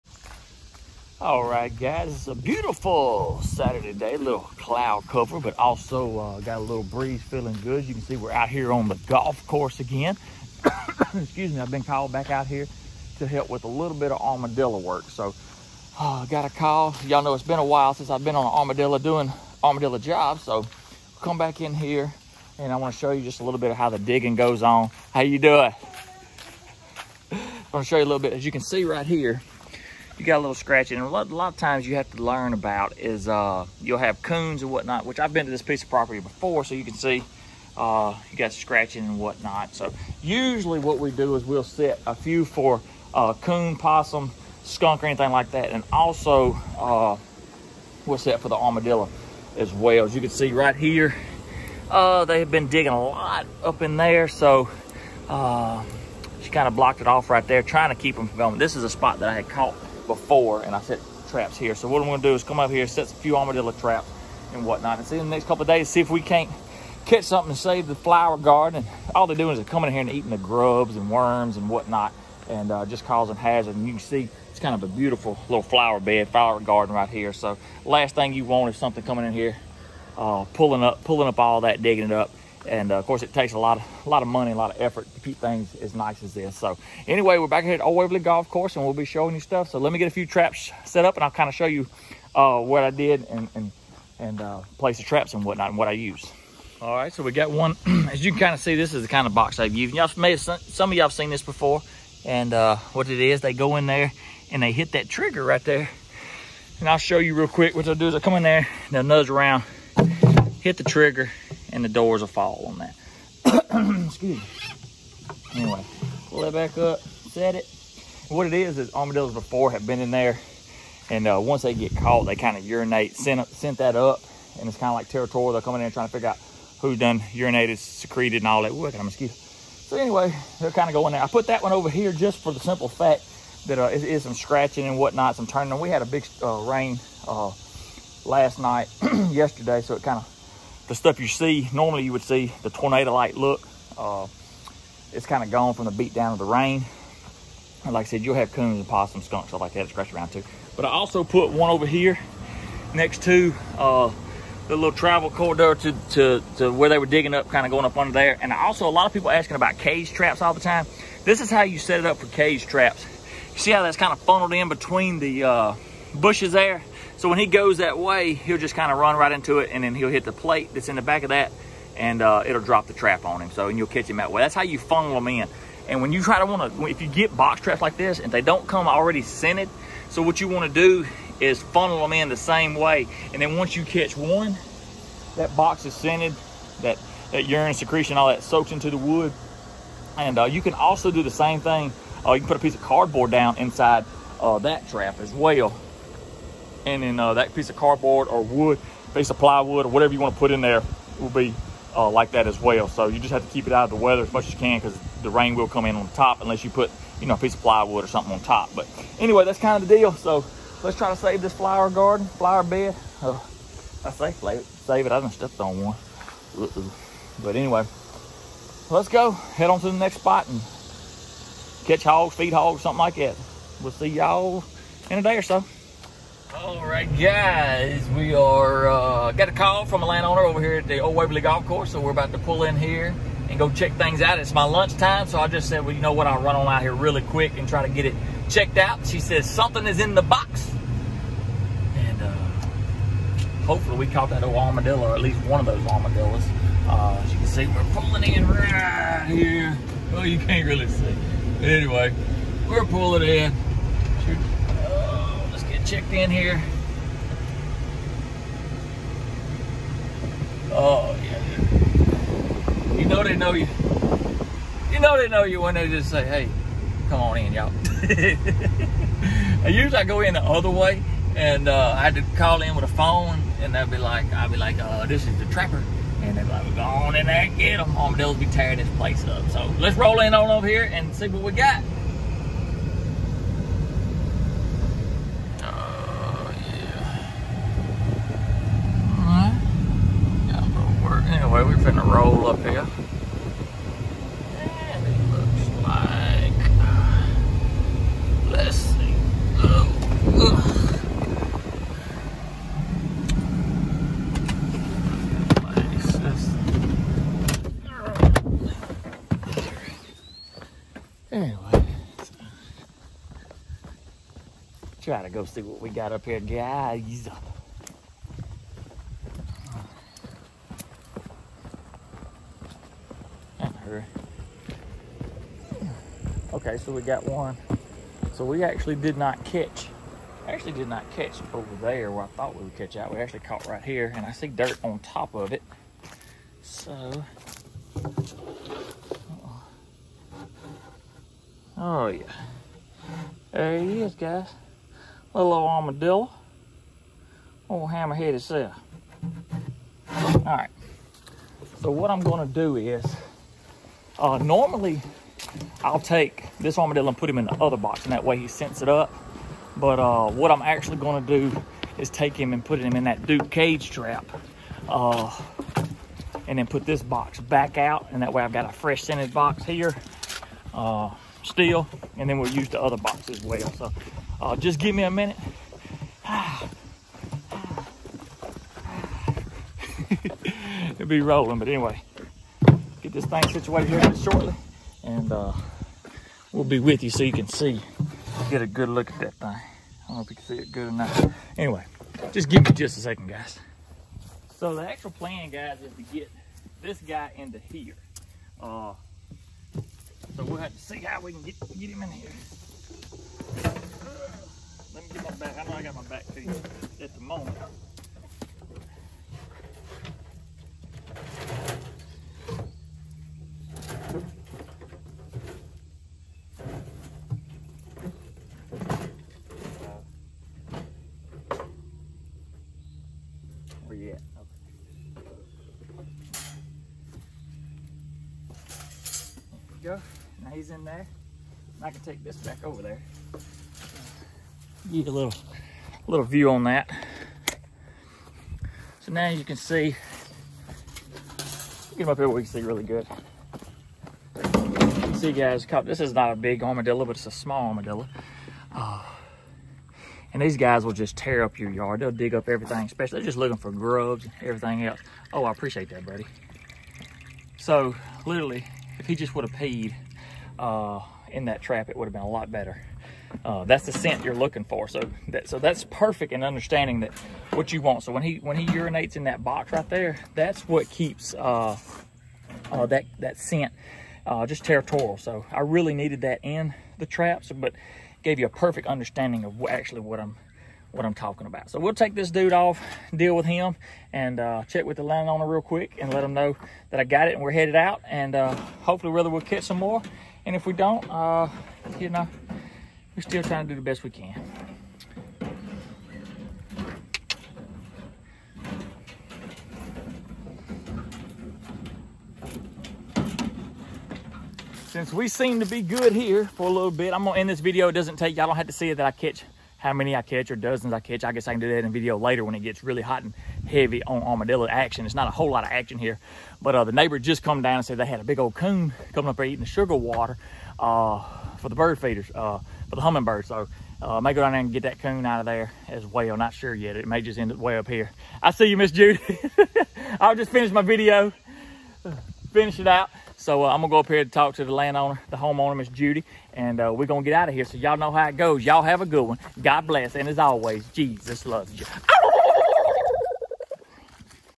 Catching them yard rooting ARMADILLOS
Today I’m trying to show how I catch those pesky armadillos from rooting up flower beds and the yard.
 How to catch armadillos is always a question I get. So sit back and learn just a little bit. Maybe lol.

Dont forget if you enjoy this channel and would like to support it hit the link below and thank y’all.